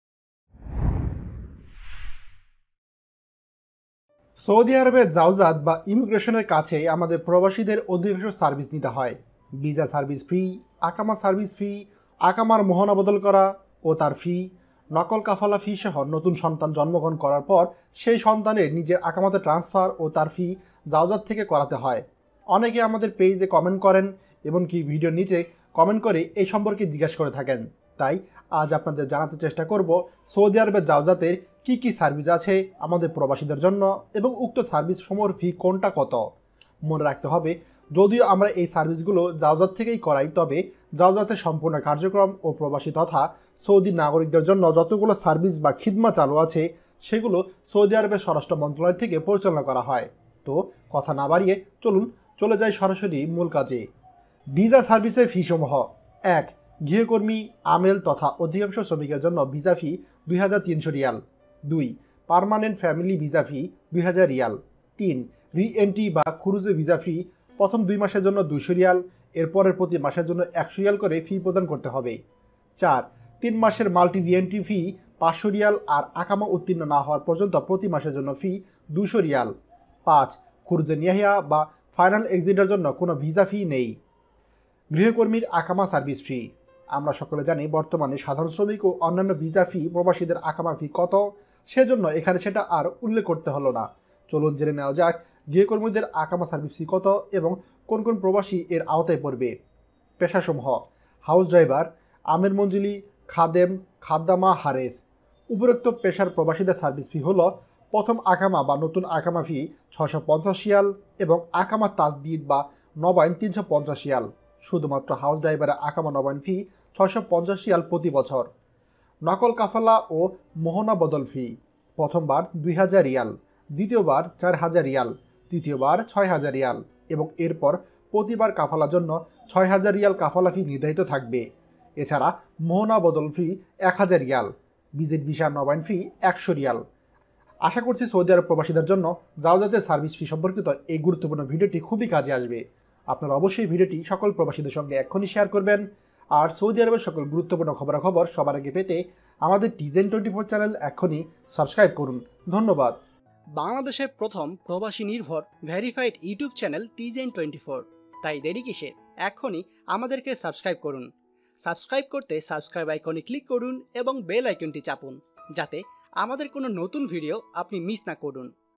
সৌদি প্রবাসীদের জন্য তথ্যবহুল একটি ভিডিও। সৌদি আরবের জাওজাত ও জাওজাতের সার্ভিস সম্পর্কে জেনে নিন।
▶ আমাদেরকে স্পন্সর করতে অথবা  বিজ্ঞাপন দিতে আমাদের ইমেইল করুন